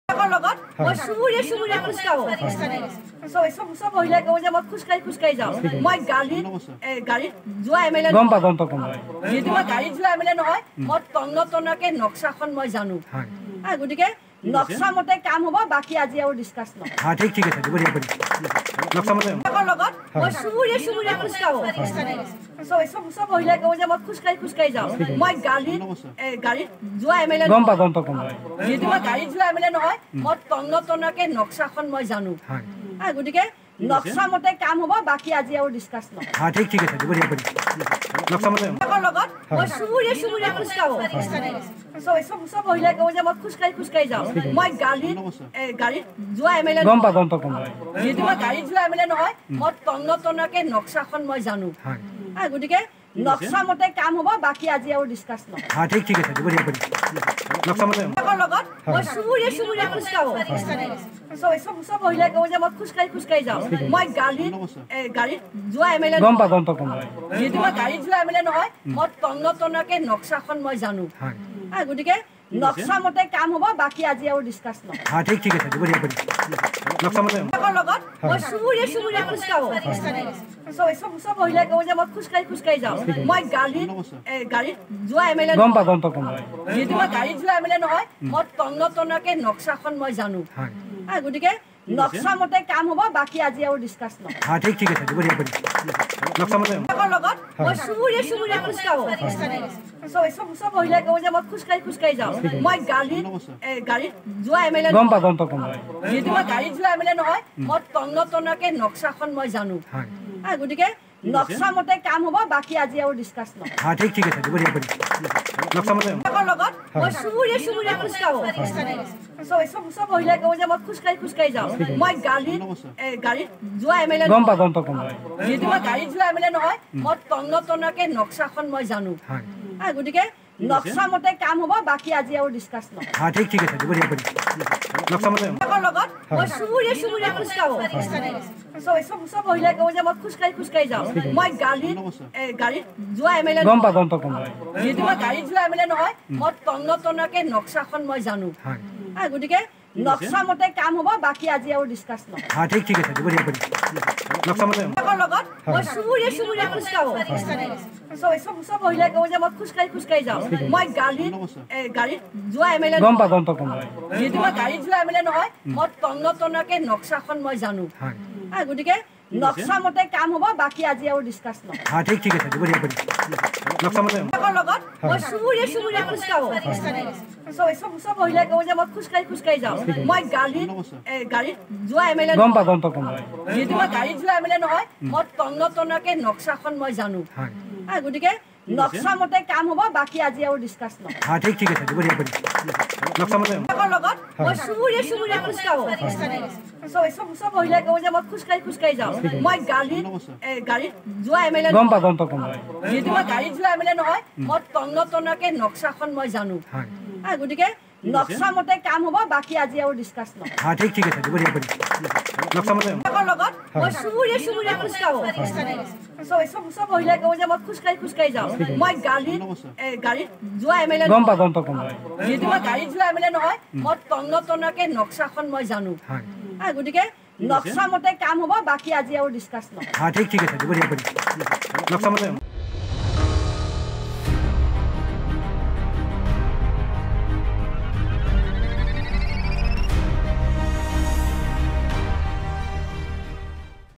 'মই গাড়ীত ঘূৰি ফুৰা MLA নহয়'
📌'কাম নক্সা মতে হব নহলে নহব'
📌হাজোত ৰাইজক অভয়বাণী সুমন হৰিপ্ৰিয়াৰ
Ishan News